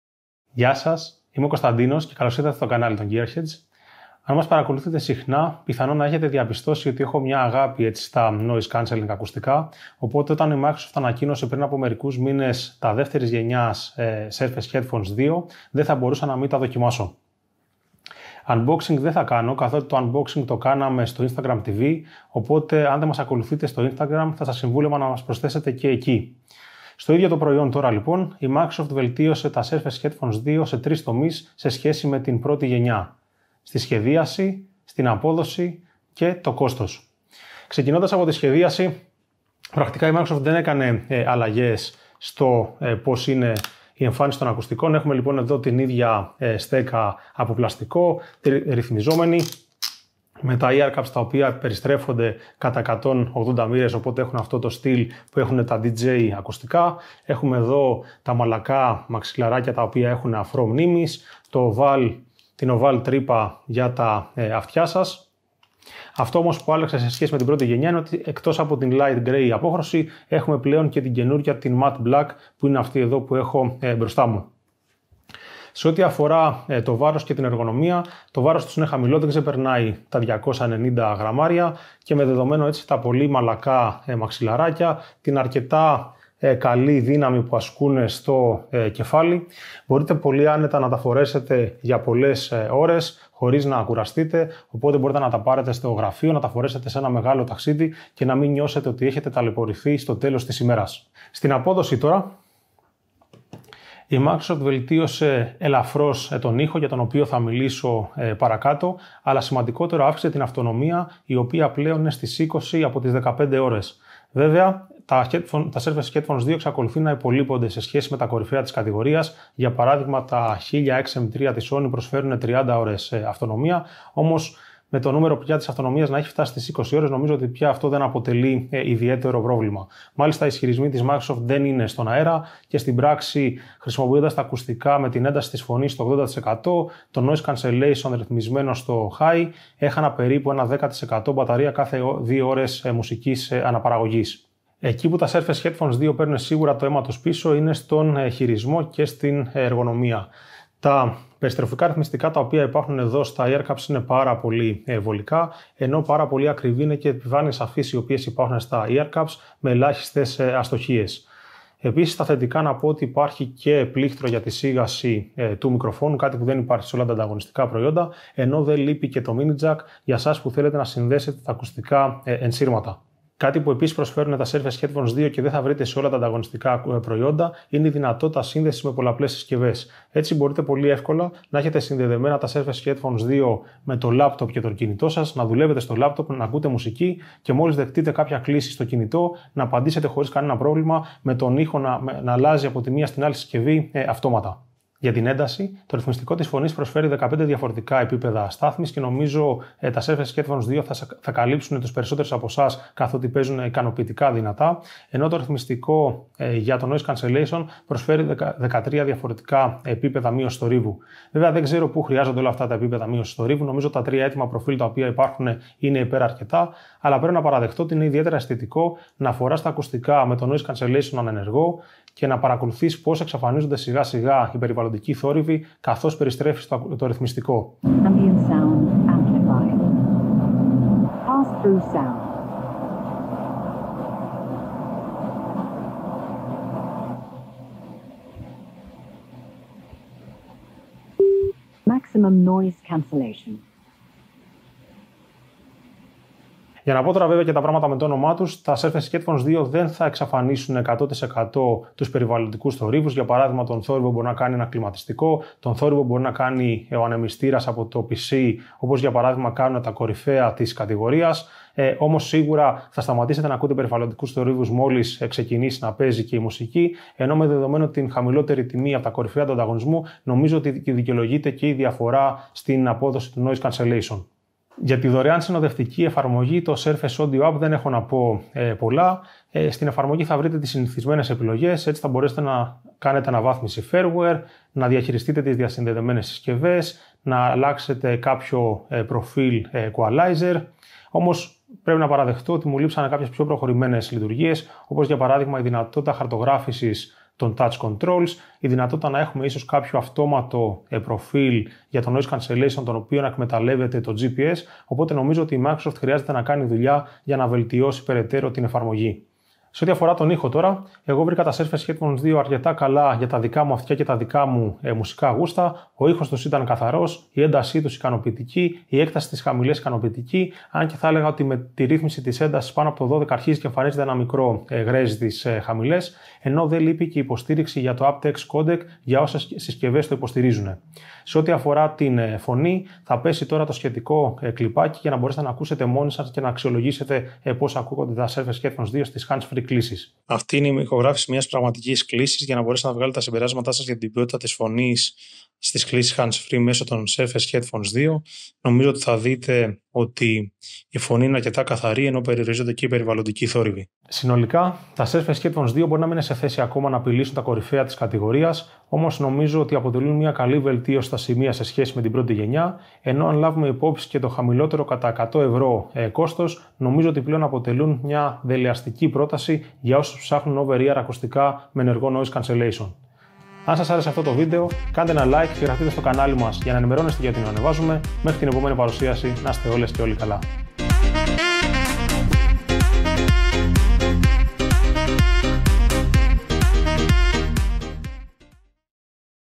Όμορφα, ήσυχα, βελτιωμένα (Microsoft Surface Heaphones 2 review) | The GearHeadz
Δοκιμάζουμε τη δεύτερη γενιά των ασύρματων noise cancelling ακουστικών της Microsoft.